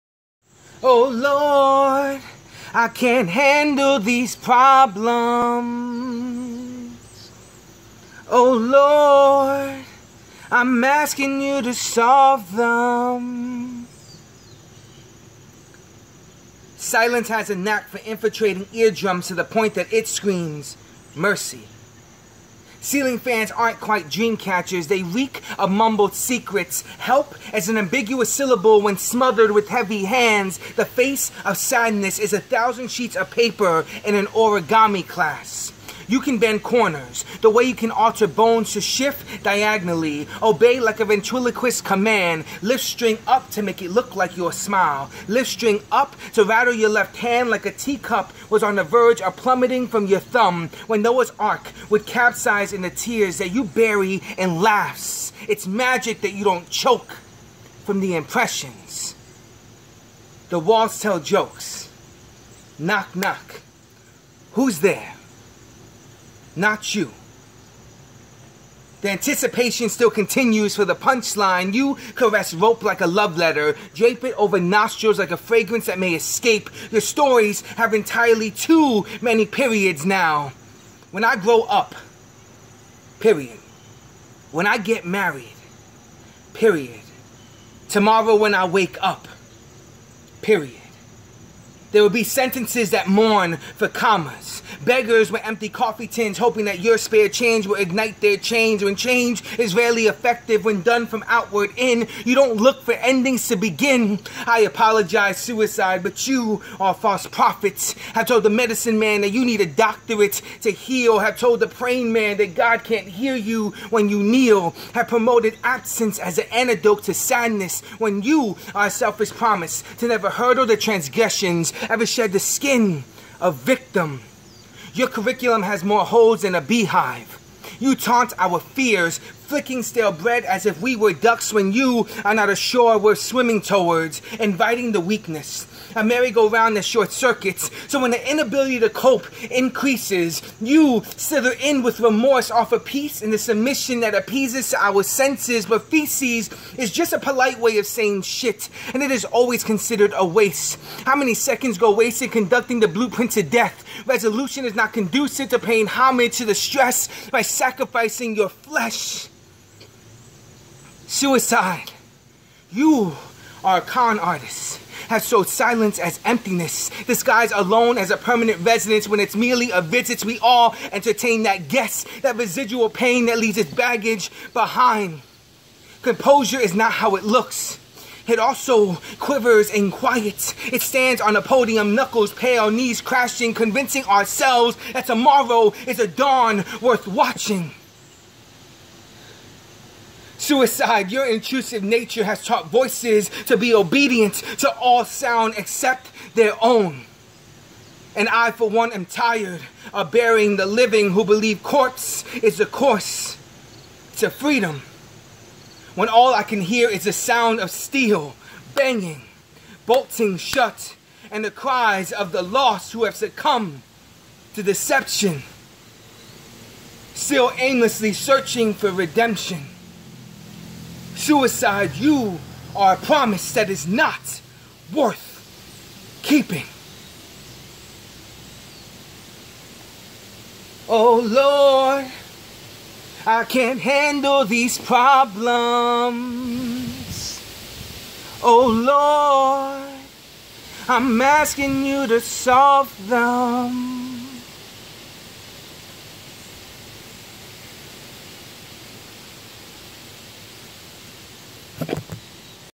Not all promises are worth keeping (Suicide Poem) by Frankie A. Soto
Written after a friend committed suicide before her 30th birthday. So many feel detached from love and God and connection they give up.

"Not all promises are worth keeping"
(C)Frankie A. Soto
Hiddenlegacy.org
National Poetry Awards
2016 Multicultural Spoken Word  of Year